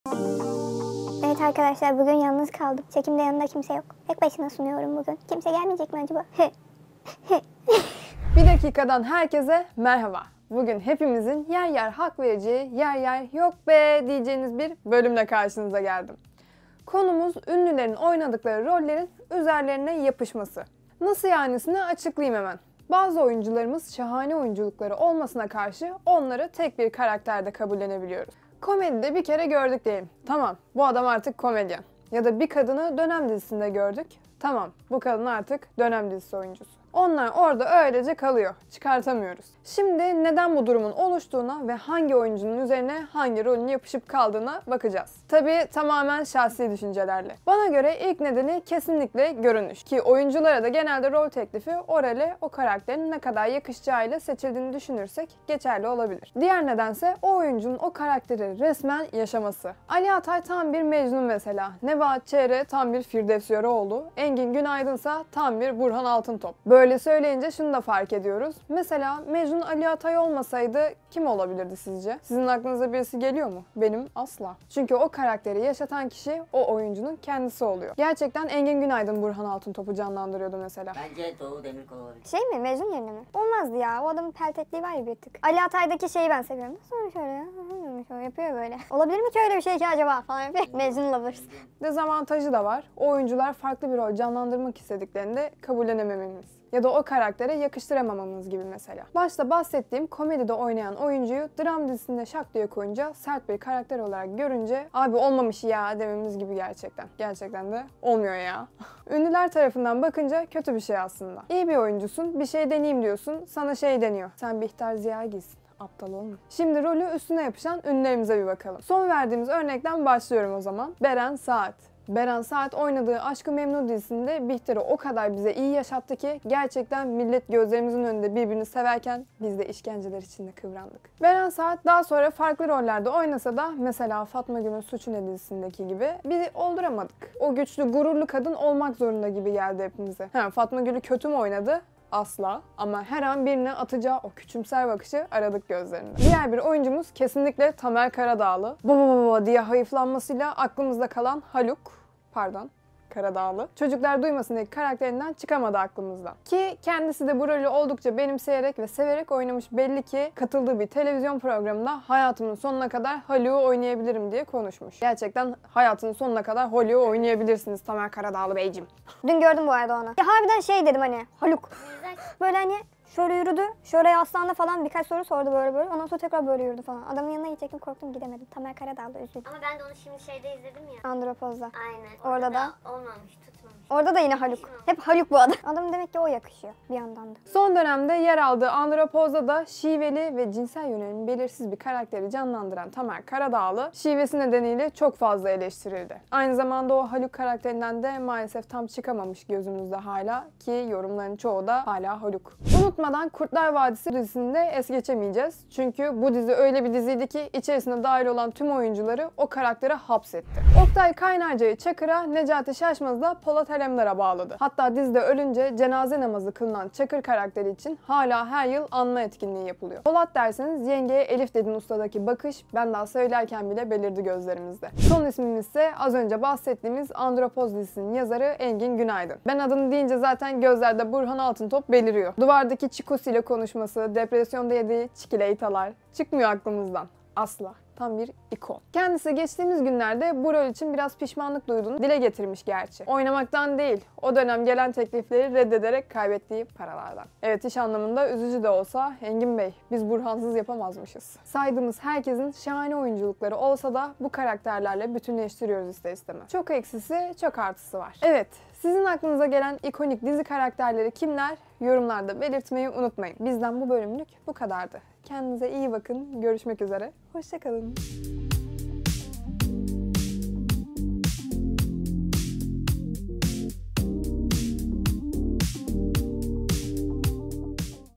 Ünlülerin üzerlerine yapışan roller: Neden onları farklı görmek istemiyoruz?
Bugün hepimizin yer yer hak vereceği, yer yer “yok beee” diyeceğiniz bir bölümle karşınıza geldim. Konumuz: ünlülerin oynadıkları rollerin üzerlerine yapışması. “Nasıl yani?”sini açıklayayım hemen. Bazı oyuncularımız şahane oyunculukları olmasına karşı onları tek bir karakterde kabullenebiliyorum. Komedide bir kere gördük diyelim. Tamam. Bu adam artık komedyen ya da bir kadını dönem dizisinde gördük. Tamam, bu kadın artık dönem dizisi oyuncusu. Onlar orada öylece kalıyor. Çıkartamıyoruz.

Şimdi neden bu durumun oluştuğuna ve hangi oyuncunun üzerine hangi rol yapışıp kaldı bunlara bakacağız. Tabii tamamen şahsi düşüncelerle…
Bana göre ilk nedeni kesinlikle görünüş. Ki oyunculara da genelde rol teklifi o role o karakterin ne kadar yakışacağı ile seçildiğini düşünürsek en geçerli neden bu oluyor.
Diğer nedense o oyuncunu o karakteri resmen yaşaması.
AliAtay tam bir Mecnun mesela, NebahatÇehre tam bir FirdevsYöreoğlu, Engin Günaydın tam bir burhan altıntop mesela…
Böyle söyleyince şunu da fark ediyoruz. Mesela mecnun Ali Atay olmasaydı kim olabilirdi? Sizin aklınıza birisi geliyor mu? Benim asla… Çünkü o karakteri yaşatan kişi o oyuncunun kendisi oluyor. Gerçekten Engin Günaydın BurhanAltıntop’u canlandırıyordu mesela.
Dezavantajı da var. O oyuncular farklı bir rol canlandırmak istediklerinde kabullenemememiz ya da o karaktere yakıştıramamamız gibi mesela…
Başta bahsettiğim komedide oynayan oyuncuyu dram dizisinde sert bir karakter olarak görünce ‘abi olmamış yeaaa’ dememiz gibi. Gerçekten… Olmuyor yeaaa…

Ünlüler tarafından bakınca kötü bir şey aslında. İyi bir oyuncusun farklı bir şey deniyim diyorsun sana şey diyor: “Sen BihterZiyagil’sin… Aptal olma…”
Şimdi rolü üstüne yapışan ünlülerimize bakalım.

Son verdiğimiz örnekten başlıyorum. Beren saat. BerenSaat oynadığı AşkıMemnu dizisinde Bihter’i o kadar bize iyi yaşattı ki. Gerçekten millet gözlerimizin önünde birbirini severken biz de işkenceler içinde kıvrandık.
Beren saat daha sonra farklı rollerde oynasa da mesela FatmagülünSuçuNe dizisinde: bir olduramadık. O güçlü gururlu kadın olmak zorunda gibi geldi hepimize. Hee… Fatmagül’ü kötü mü oynadı? Asla. Ama her an birine atacağı o küçümser bakışı aradık gözlerinde.

Diğer bir oyuncumuz kesinlikle TamerKaradağlı. “Babababa” diye hayıflanmasıyla aklımızda kalan haluk. Pardon Karadağlı. ÇocuklarDuymasın’daki karakterinden çıkamadı aklımızdan. Ki kendisi de bu rolü oldukça benimseyerek ve severek oynamış belli. Kendisi de katıldığı bir televizyon programında "hayatımın sonuna kadar Haluk’u oynayabilirim" diye konuşmuştu.
Son dönemde yer aldığı Andropoz’da şiveli ve cinsel yönelimi belirsiz bir karakteri oynadığını gördük. En çok eleştirildiği nokta şivesi olurken diğer taraftan da yok ya hala haluk abimiz bu yorumları da geldi.

Unutmadan KurtlarVadisiPusu dizisini es geçemeyeceğim. Çünkü bu dizi öyle bir diziydi ki içerisine dâhil olan tüm oyuncuları o karakterlere hapsetti. OktayKaynarca’yı Çakır’a, NecatiŞaşmaz’ı PolatAlemdar’a bağladı. Hatta dizide ölünce cenaze namazı kılınan çakır karakteri için hala her yıl anma etkinliği yapılıyor. Polat alemdar deseniz... “Yengeye elif dedin usta”daki bakış ben daha söylerken bile belirdi gözlerde.

Son ismimizde az önce bahsettiğimiz Andropoz dizisinin yazarı EnginGünaydın. Ben adını deyince zaten gözlerde burhan beliriyor. Duvardaki Çiko’suyla konuşması, depresyonda yediği ‘çikileytalar’ çıkmıyor aklımızdan.

Kendisi geçtiğimiz günlerde bu rol için biraz pişmanlık duyduğunu dile getirmişti gerçi. Oynamaktan değil, o dönem gelen teklifleri reddederek kaybettiği paralardan… Evet, iş anlamında üzücü de olsa… Engin bey… Biz Burhan’sız yapamazmışız…

Saydığımız herkesin şahane oyunculukları olsa da bu karakterlerle bütünleştiriyoruz ister istemez… Çok eksisi çok artısı var… Evet… Sizin aklınıza gelen ikonik dizi karakterleri kimler? Yorumlarda belirtmeyi unutmayın.